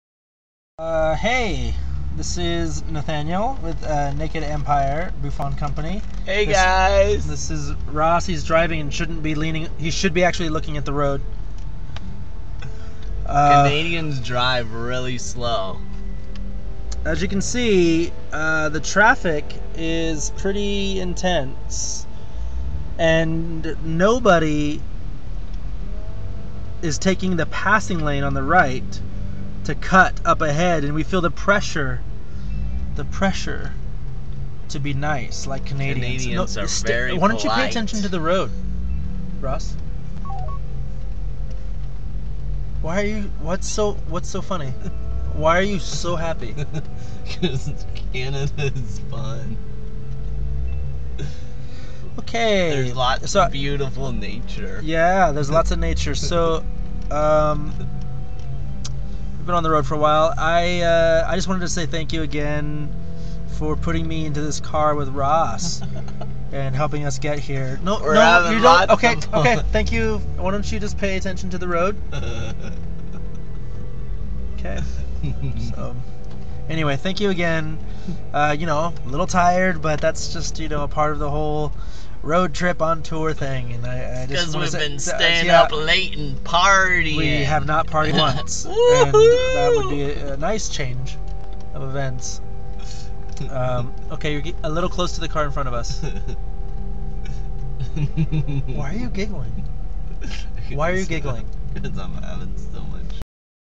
Thanking our supporters while on the road.
August 2013. Naked Empire Bouffon Company's road trip from San Francisco to Edmonton to get to the Edmonton Fringe Festival. We're bringing our show, You Killed Hamlet, or Guilty Creatures Sitting At A Play.